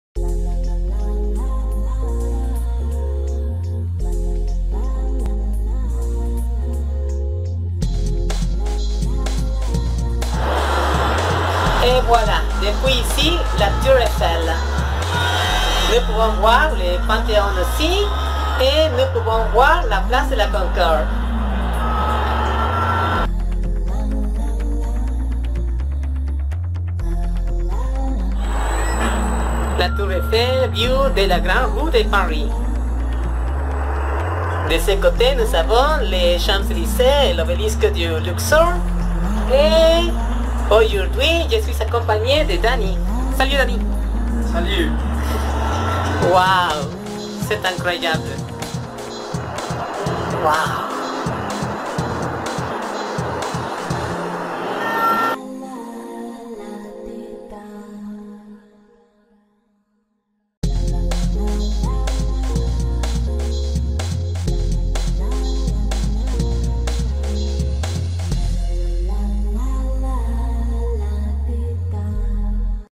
La Grande Roue de Paris/ La Gran Noria de París
Une vue de Paris depuis la Grande Roue avec la Place de la Concorde, le Panthéon et les Champs Élysées mais principalement la belle Tour Eiffel.

Una vista de París desde la Gran Noria con la Plaza de la Concordia, el Panteón y los Campos Elíseos pero, sobre todo, la hermosa Torre Eiffel.